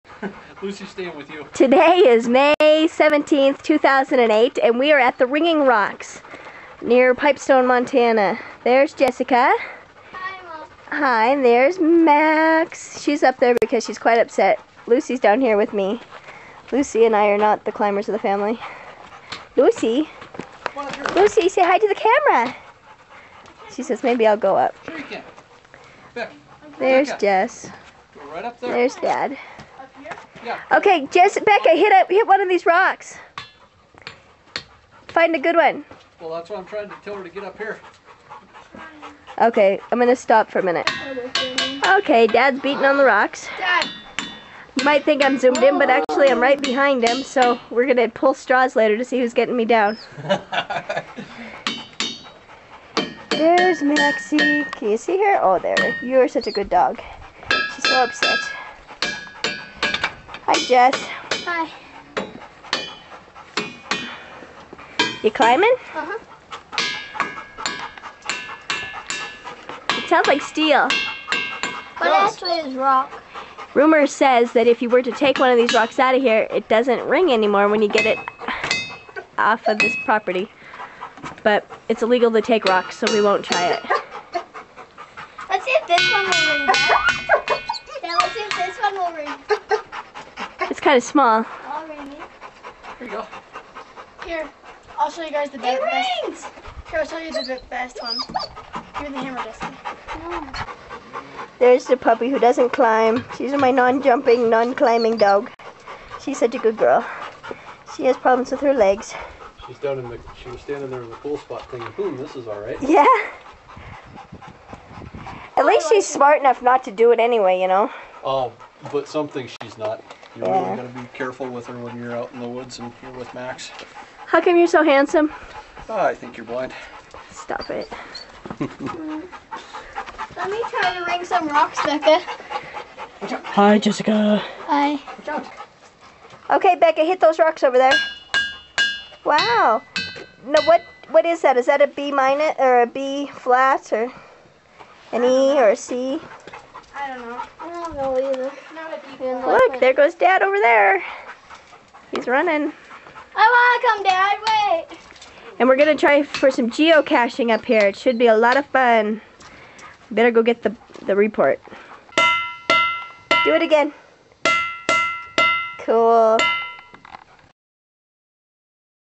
Montana Ringing Rocks
This unique geological formation is located approximately 18 miles east of Butte and north of I-90. The rocks in this unique geologic area chime when tapped lightly with a hammer. It is believed that the ringing is a combination of the composition of the rock and the way the joining patterns have developed as the rocks have eroded away, if a boulder is removed from the pile, it doesn't ring.